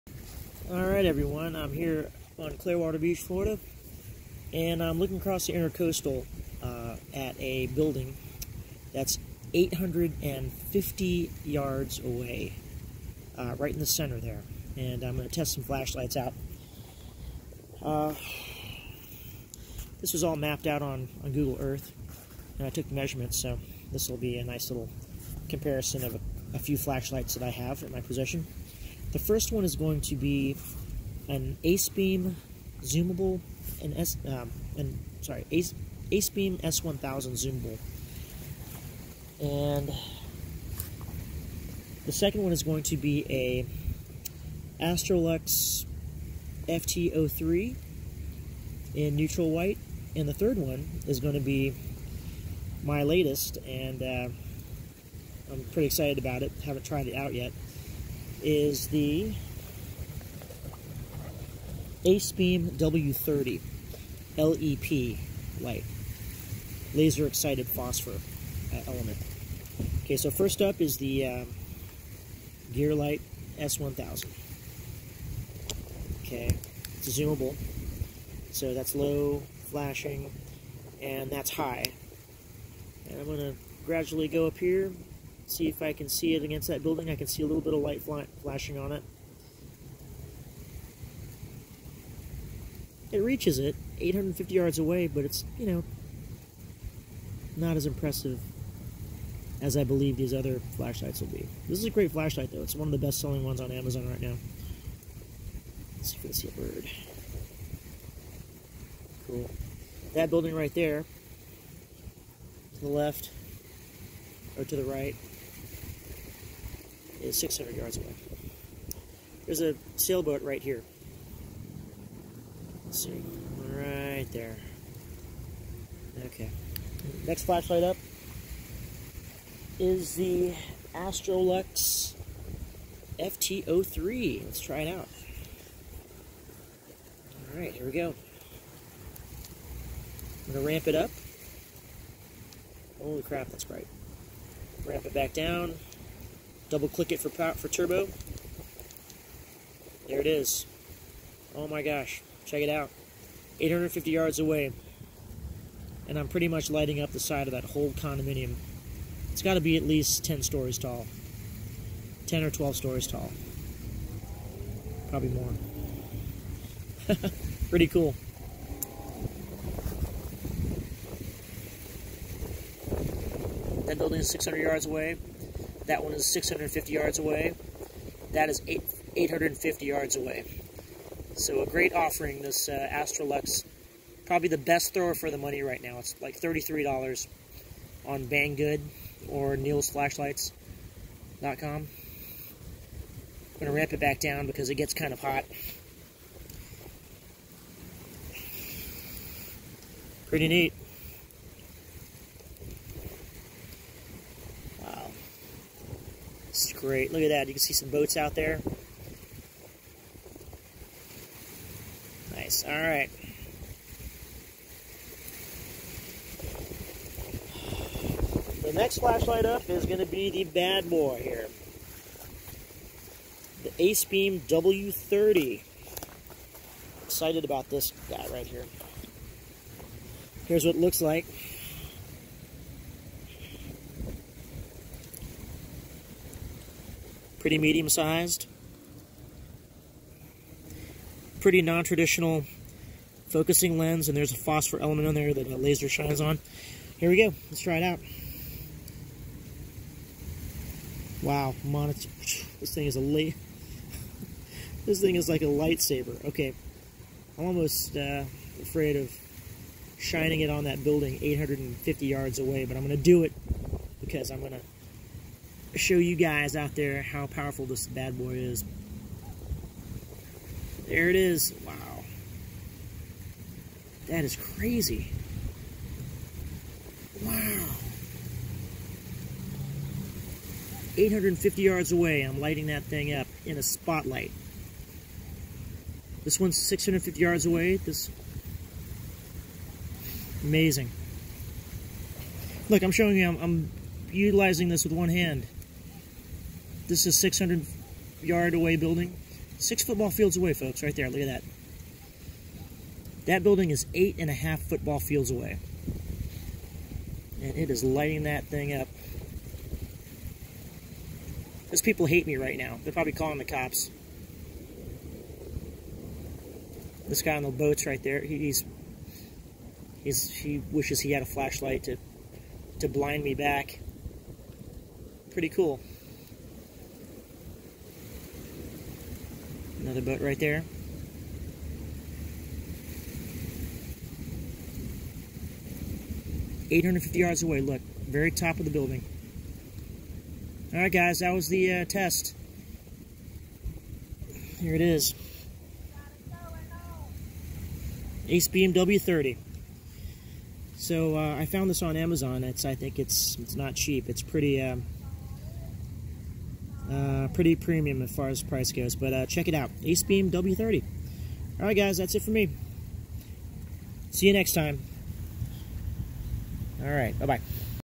Flashlight 850 yard Beam Test! Gearlight S1200 vs. Astrolux FT03 vs. Acebeam W30
Flashlight 850yard distance Beam Test between the Gearlight S1200 vs. Astrolux FT03 vs. Acebeam W30..

Gearlight S1000:
 Acebeam W30: